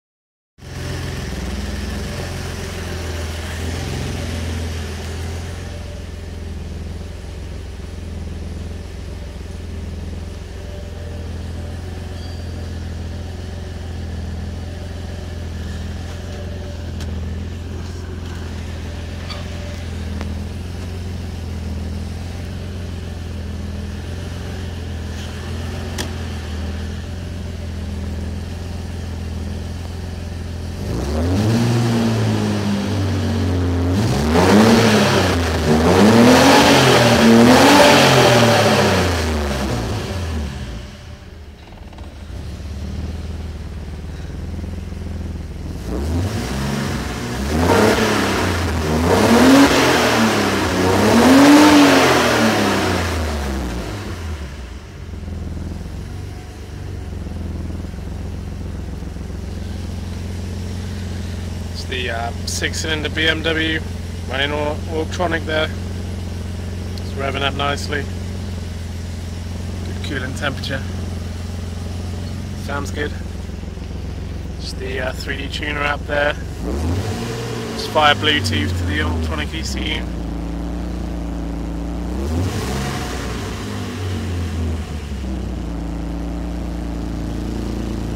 Augment Automotive AugTronic BMW E28 M5
Our AugTronic ECU running a BMW E28 M5 on the factory wiring loom.